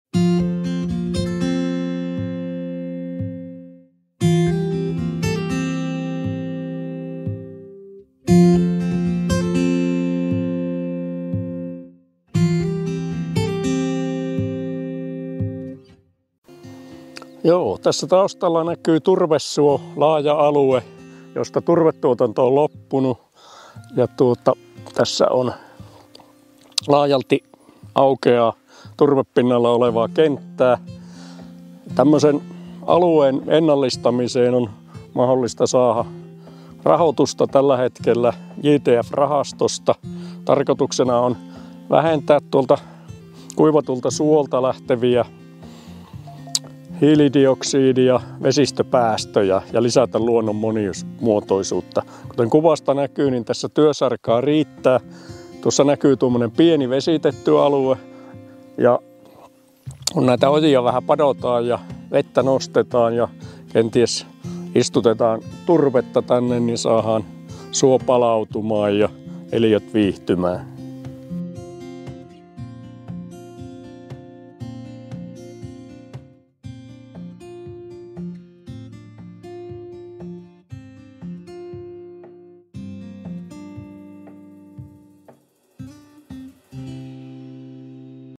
JTF-rahoitus turvetuotantoalueiden ennallistamiseen ja jälkikäyttöön
Turvetuotannosta poistuvat suot tarjoavat ennallistamisen jälkeen kodin monille lajeille ja virkistyskäyttömahdollisuuksia ihmisille. 🦆 🦗 🌿

➡ Hae ELY-keskukselta JTF-rahoitusta turvesoiden ennallistamiseen ja jälkikäyttöön Pohjois-Suomessa! Hakuilmoitus löytyy eura2021.fi-järjestelmästä.